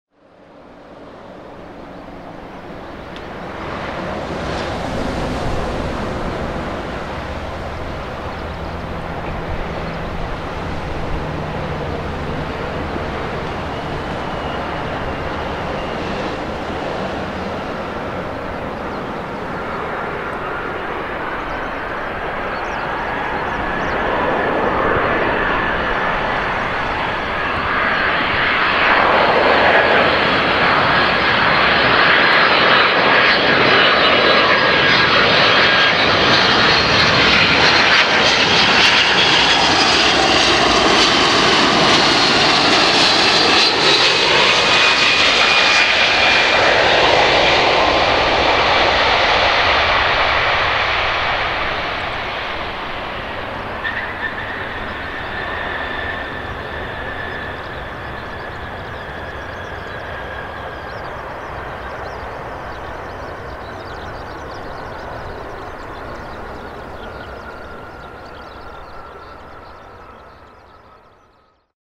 ✈Jet Asia Airways B767 HS-JAE landing @Narita Airport rwy16L(成田空港)
Apr.27, 2015.
成田空港 16Lエンドトにて。
■movie camera : JVC Everio Gz-E565
■IC recorder : sony ICD-UX523
■microphone : Audio technica AT9913